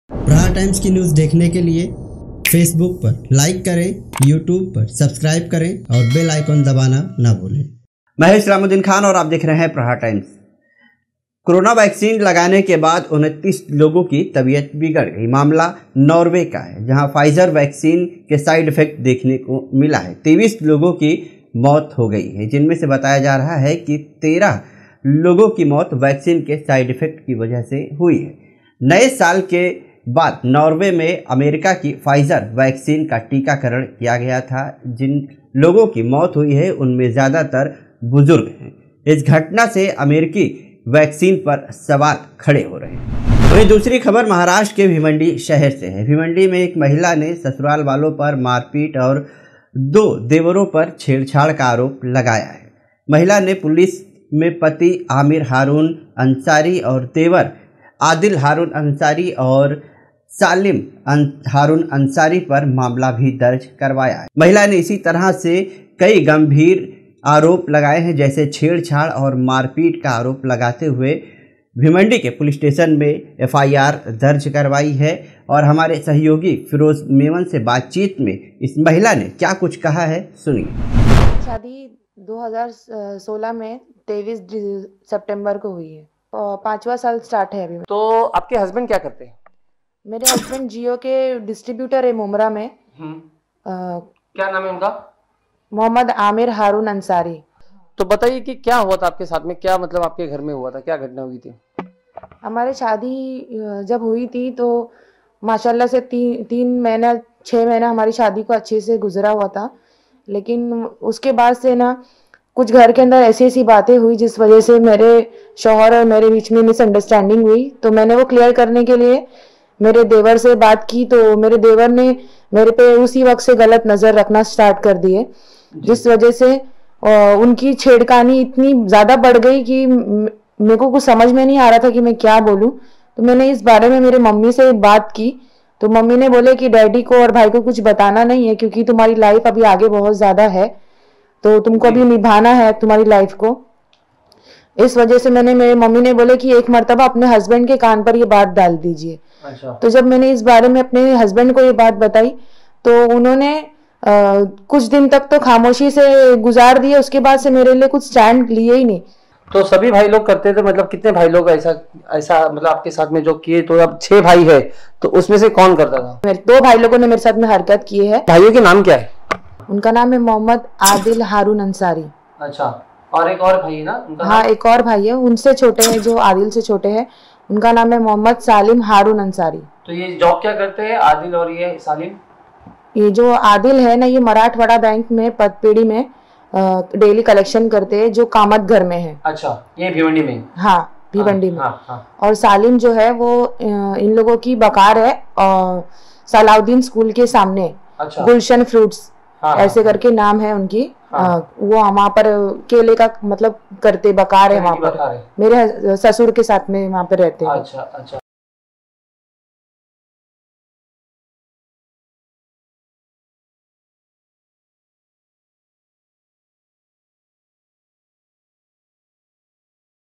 Corona Vaccine से 23 लोगो की मौत भाभी ने देवर पर छेड़छाड़ का लगाया आरोप || Aapka Prahar Times
corona vaccine से २३ लोगो की मौत भाभी ने देवर पर छेड़छाड़ का लगाया आरोप